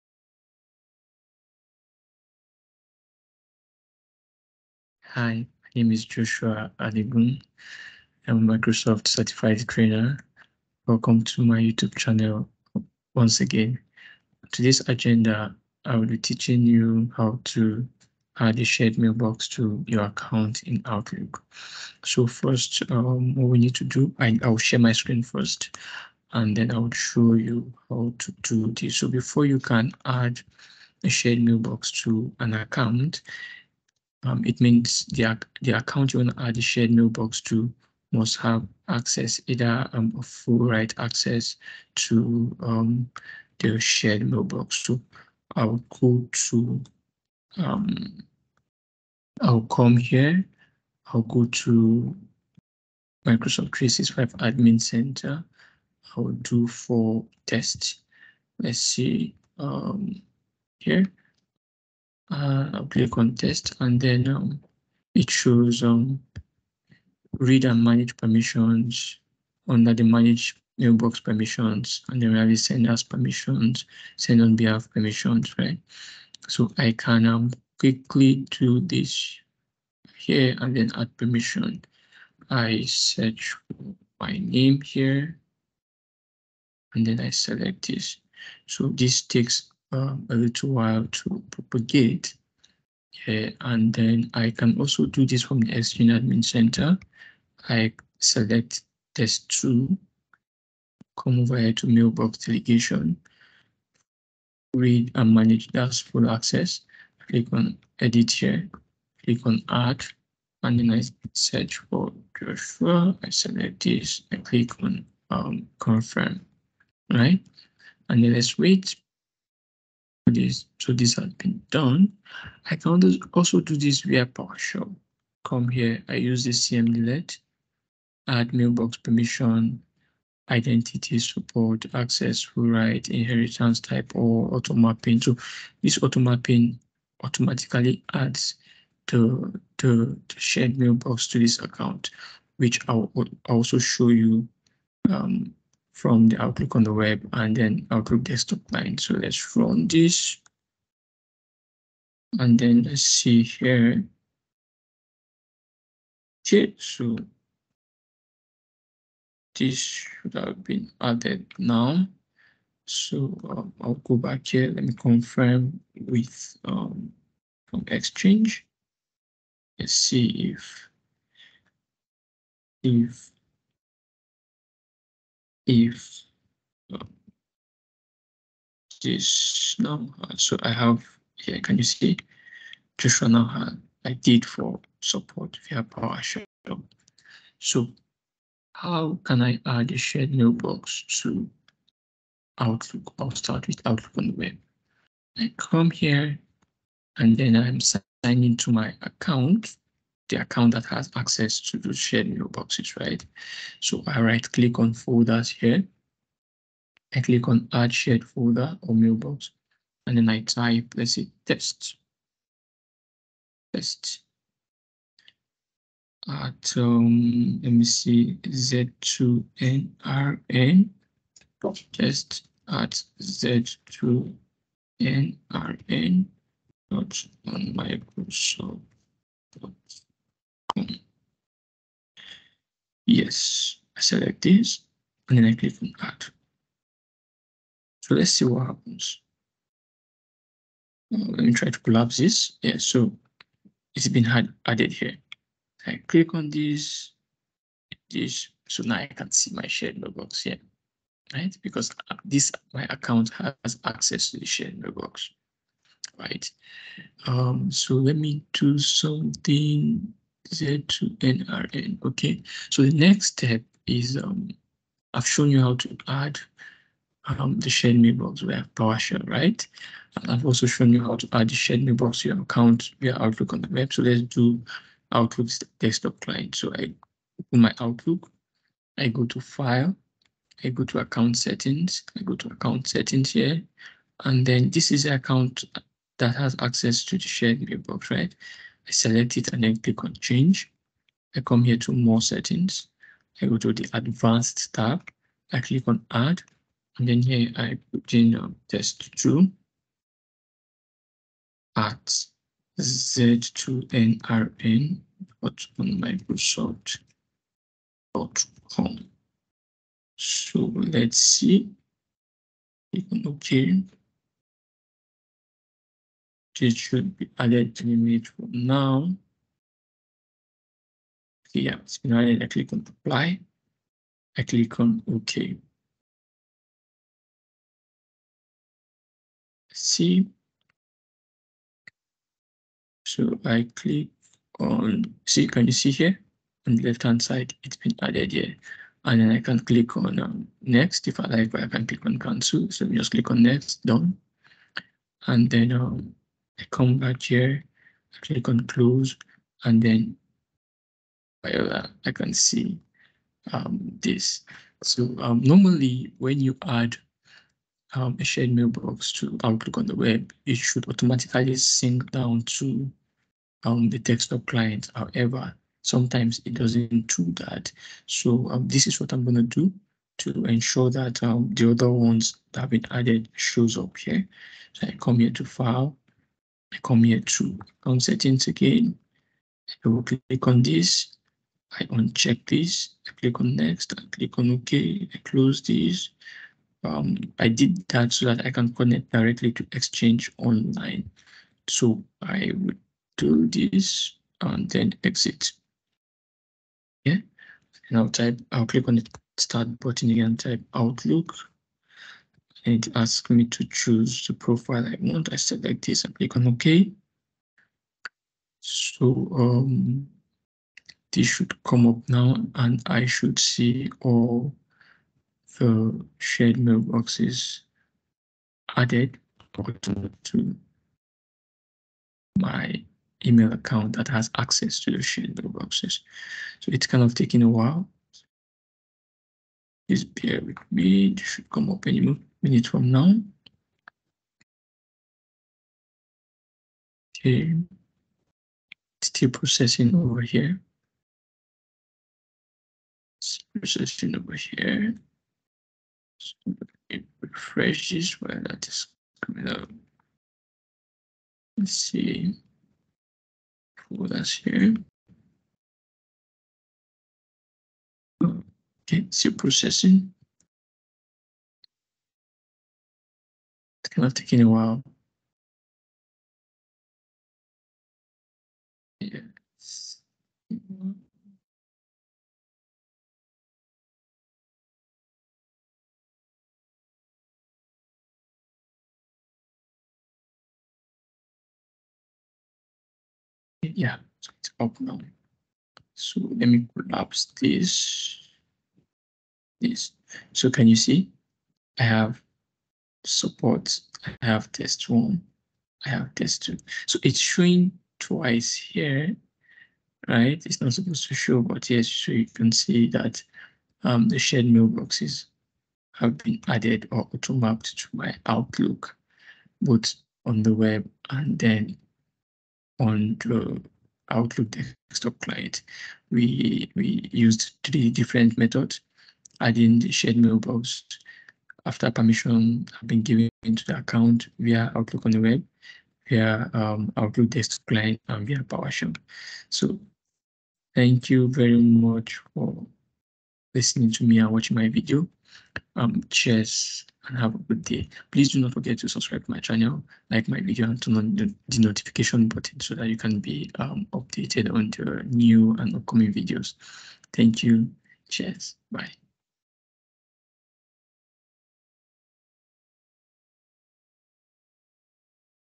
How to add a shared mailbox to your account in outlook in 2023
"Unlock the potential for seamless collaboration and improved communication within your organization by mastering the art of adding a shared mailbox to your personal account in Outlook 2023. In this comprehensive tutorial, I will walk you through each step of the process, providing clear and easy-to-follow instructions.

Discover the efficient process of accessing and managing shared email resources, enabling you to collaborate effectively with colleagues and streamline communication within your organization. Whether you're a professional seeking enhanced teamwork capabilities or an administrator responsible for mailbox configurations, this guide will empower you to effortlessly add a shared mailbox to your Outlook 2023 account, enhancing your email management experience.

Shared mailboxes are an invaluable tool for teams and departments looking to enhance their efficiency and streamline their email communications. By adding a shared mailbox to your Outlook 2023 account, you gain the ability to access and manage emails and resources collectively. This means improved teamwork, centralized email access, and simplified coordination.

My guide is designed for both individuals looking to enhance their productivity and IT administrators responsible for mailbox configurations. You will learn how to effortlessly integrate shared mailboxes into your Outlook 2023 environment, making it easier than ever to collaborate, share information, and ensure everyone is on the same page.

Don't miss out on the opportunity to make your workday more efficient and your team more connected. Follow our step-by-step instructions, and soon you'll be on your way to harnessing the full potential of shared mailboxes within Outlook 2023, unlocking a world of collaborative possibilities."